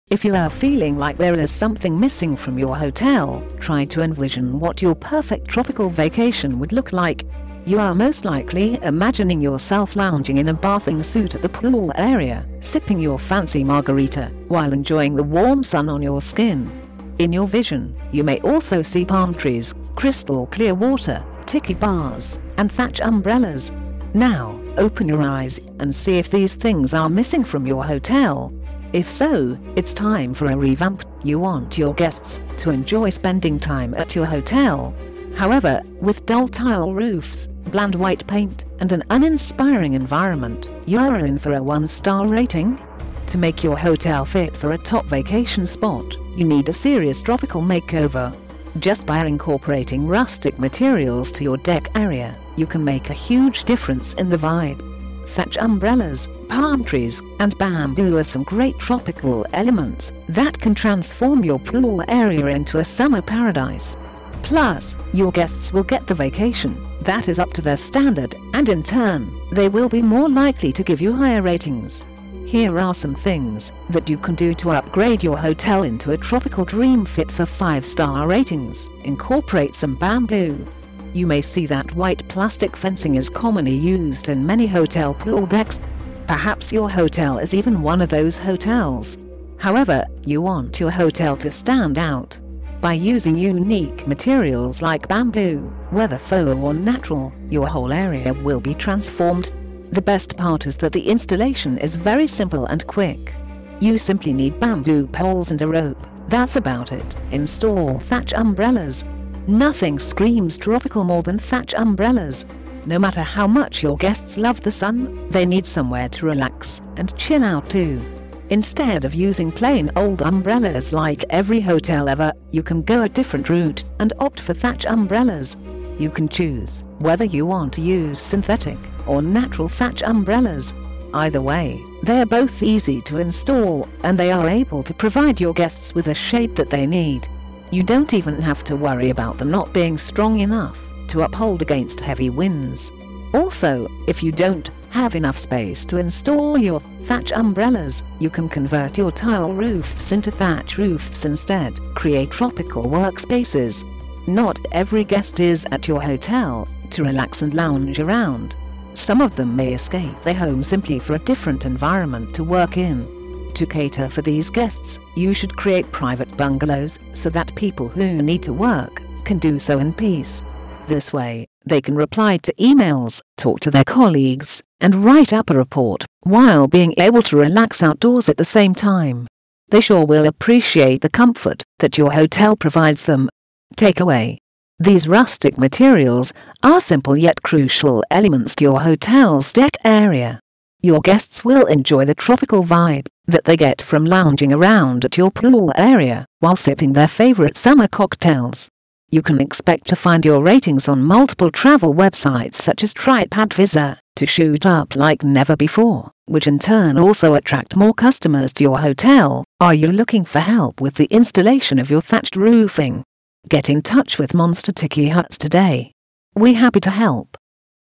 How to Improve Your Hotel Rating With Thatched Umbrellas & Roofing | MONSTER TIKI HUTS
If you’re feeling like there is something missing from your hotel, try to envision what your perfect tropical vacation would look like. You’re most likely imagining yourself lounging in a bathing suit at the pool area, sipping your fancy margarita while enjoying the warm sun on your skin. In your vision, you may also see palm trees, crystal clear water, tiki bars, and thatch umbrellas. Now, open your eyes and see if these things are missing from your hotel. If so, it’s time for a revamp!
You want your guests to enjoy spending time at your hotel. However, with dull tile roofs, bland white paint, and an uninspiring environment, you’re in for a one-star rating. To make your hotel fit for a top vacation spot, you need a serious tropical makeover.
Just by incorporating rustic materials to your deck area, you can make a huge difference in the vibe. Thatch umbrellas, palm trees, and bamboo are some great tropical elements that can transform your pool area into a summer paradise! Plus, your guests will get the vacation that is up to their standard, and in turn, they will be more likely to give you higher ratings. Here are some things that you can do to upgrade your hotel into a tropical dream fit for five-star ratings!

Incorporate Some Bamboo
You may see that white plastic fencing is commonly used in many hotel pool decks. Perhaps your hotel is even one of those hotels. However, you want your hotel to stand out. By using unique materials like bamboo – whether faux or natural – your whole area will be transformed. The best part is that the installation is very simple and quick! You simply need bamboo poles and a rope – that’s about it!

Install Thatch Umbrellas
Nothing screams tropical more than thatch umbrellas! No matter how much your guests love the sun, they need somewhere to relax and chill out, too. Instead of using plain old umbrellas like every hotel ever, you can go a different route and opt for thatch umbrellas. You can choose whether you want to use synthetic or natural thatch umbrellas. Either way, they are both easy to install and they are able to provide your guests with a shade that they need. You don’t even have to worry about them not being strong enough to uphold against heavy winds. Also, if you don’t have enough space to install your thatch umbrellas, you can convert your tile roofs into thatch roofs instead!

Create Tropical Workspaces
Not every guest is at your hotel to relax and lounge around. Some of them may escape their home simply for a different environment to work in. To cater for these guests, you should create private bungalows so that people who need to work can do so in peace. This way, they can reply to emails, talk to their colleagues, and write up a report while being able to relax outdoors at the same time! They sure will appreciate the comfort that your hotel provides them.

Takeaway
These rustic materials are simple yet crucial elements to your hotel’s deck area. Your guests will enjoy the tropical vibe that they get from lounging around at your pool area while sipping their favorite summer cocktails. You can expect to find your ratings on multiple travel websites such as TripAdvisor to shoot up like never before, which in turn also attract more customers to your hotel!
Are you looking for help with the installation of your thatched roofing? Get in touch with Monster Tiki Huts today! We’re happy to help.